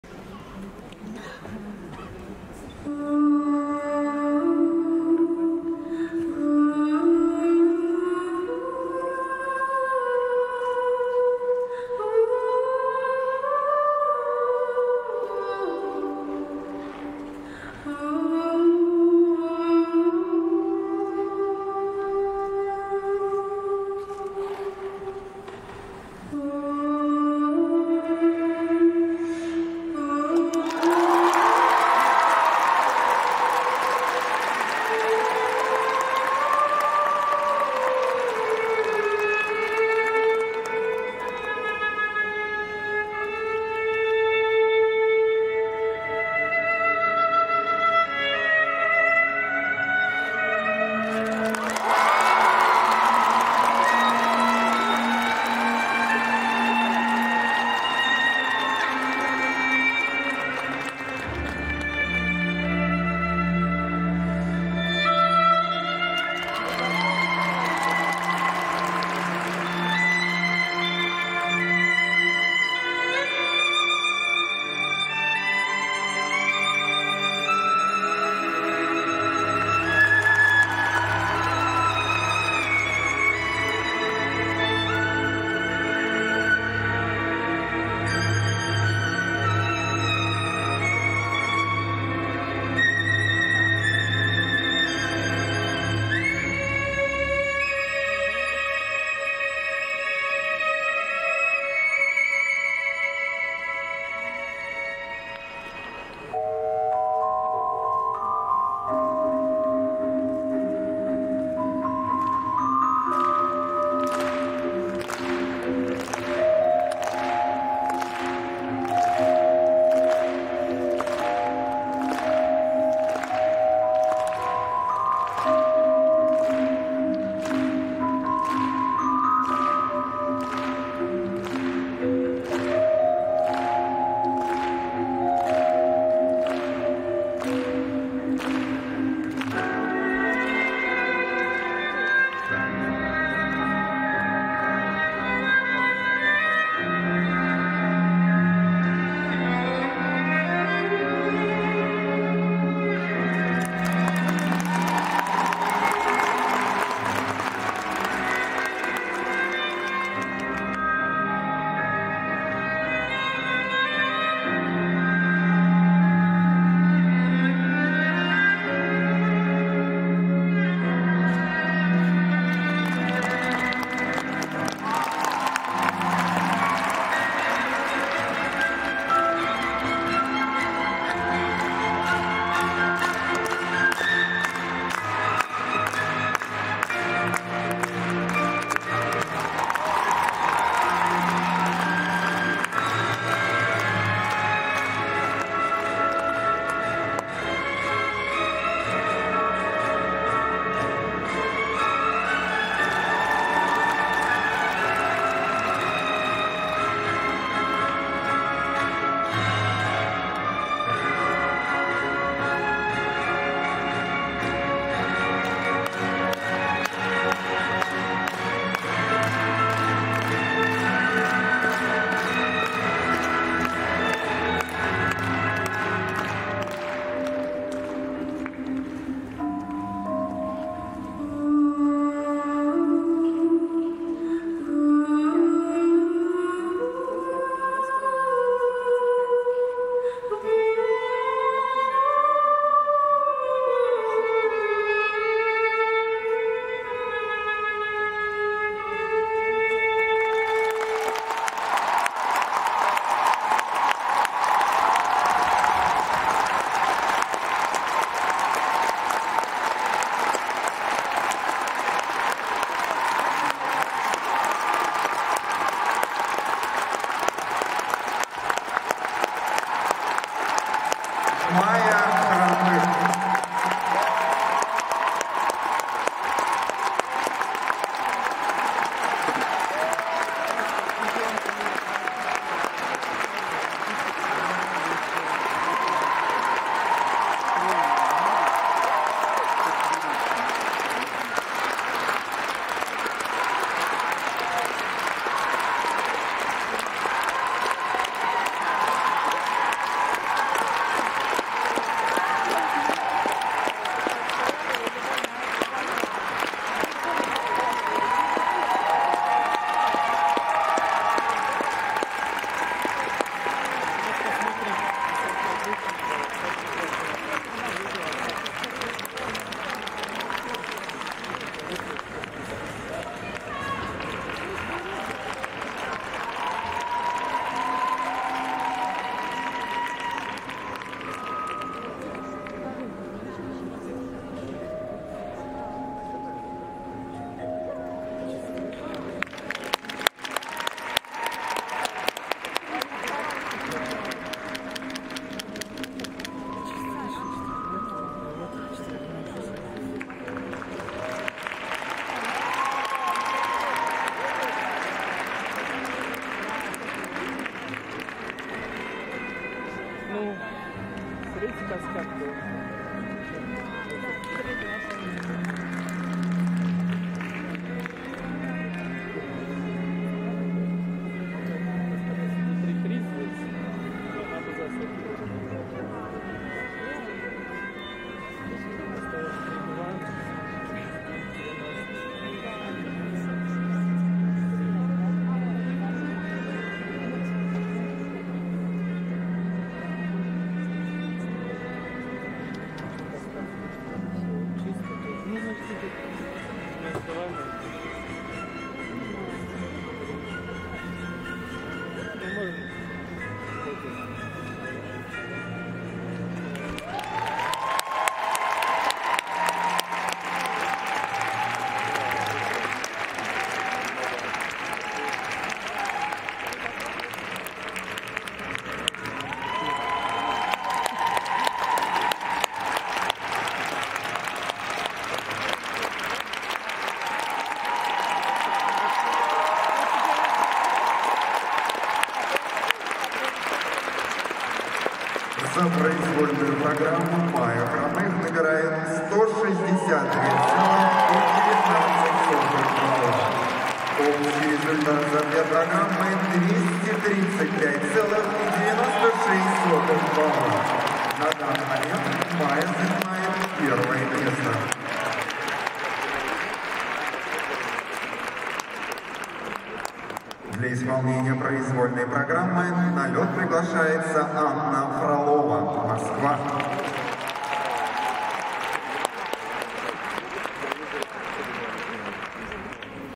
Maiia Khromykh - Russian Cup Final - FS / Майя Хромых - Финал Кубка России 2021 - ПП - 28.02.2021
Майя Хромых, Финал Кубка России по фигурному катанию 2021,  произвольная программа, 28 февраля 2021, Москва.